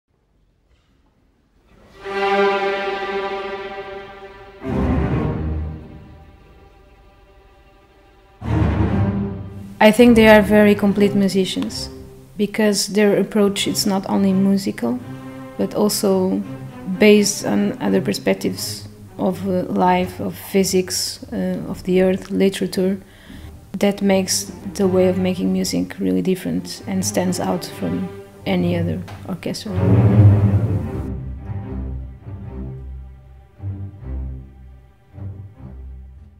MCO Academy 2016: Teaser
50 Academy students. 54 Mahler Chamber Orchestra musicians.
One choir. Two soloists.
One conductor. Three stages.
And one work to unite them all.
Here is a teaser on our Academy project in February.